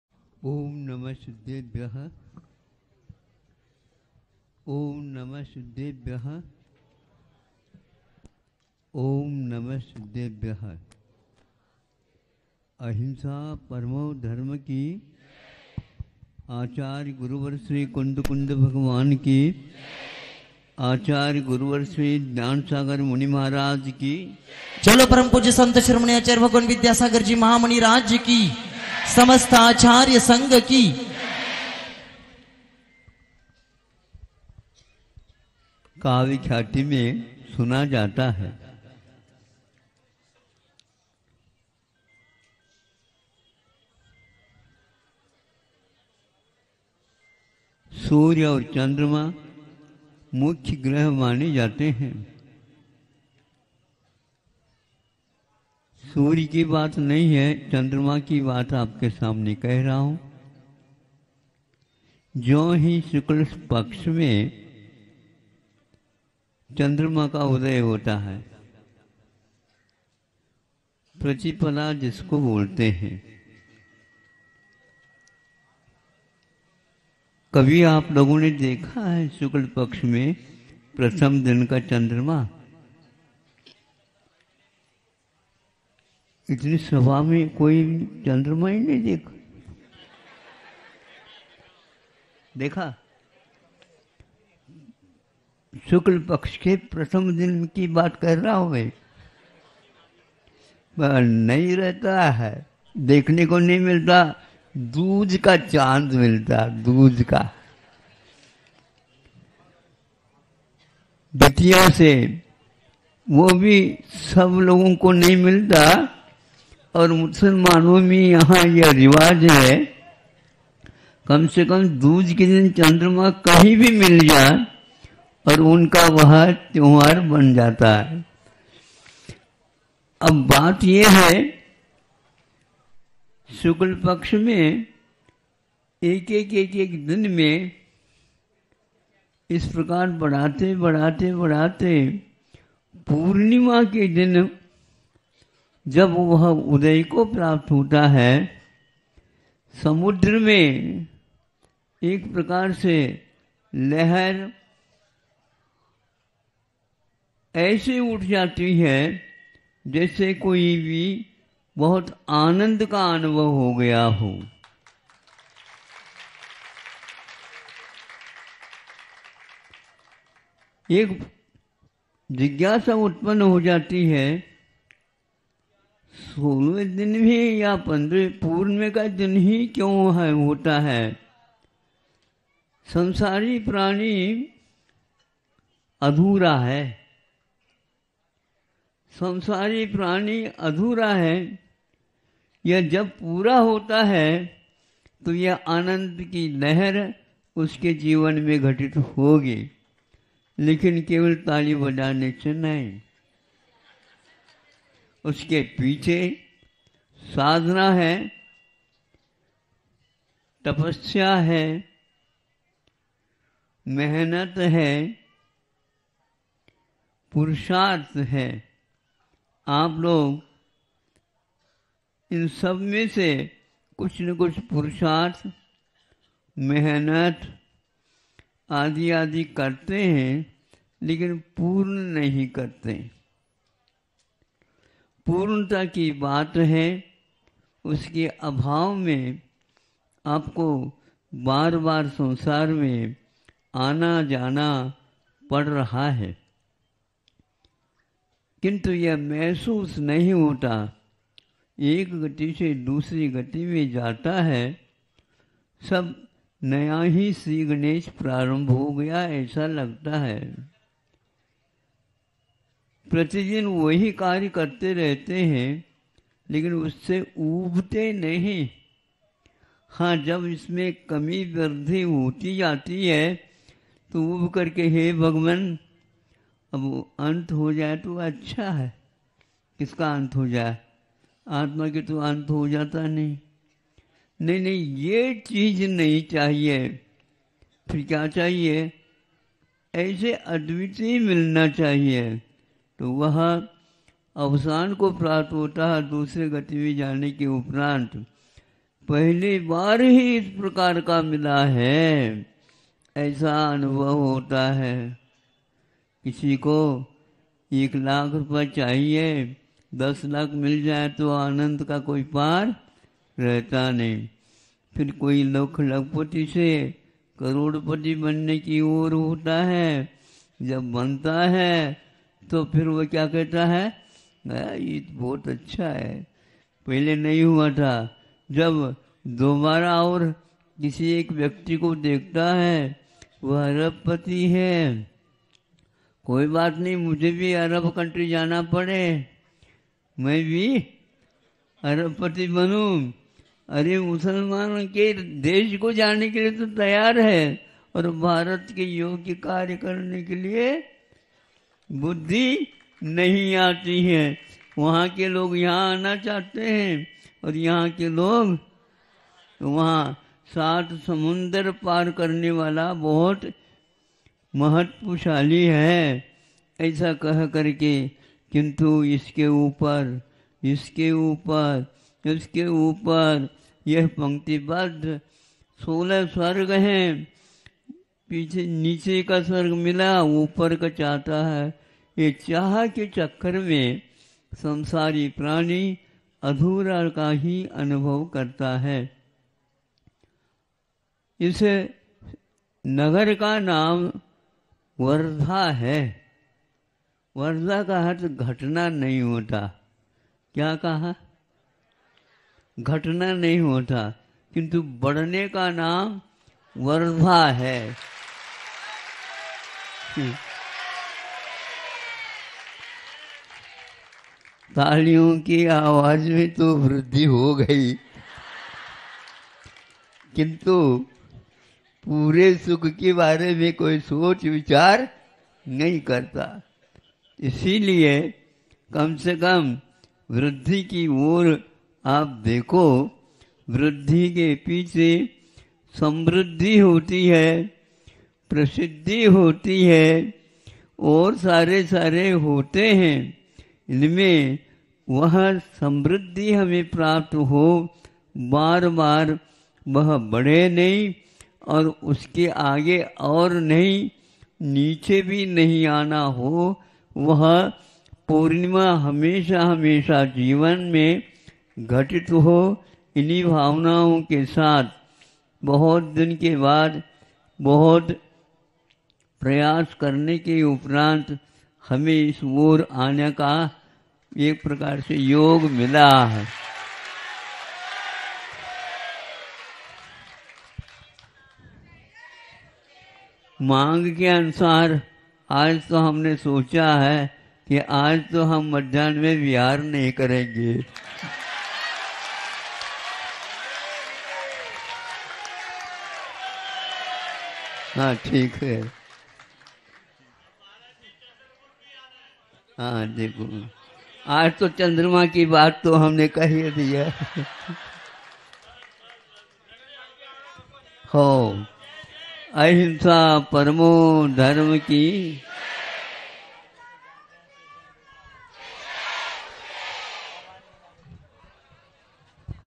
9 जनवरी 2023 आचार्य श्री विद्यासागर जी महाराज प्रवचन (वर्धा)
वर्धा अर्थात सत कार्य की वृद्धि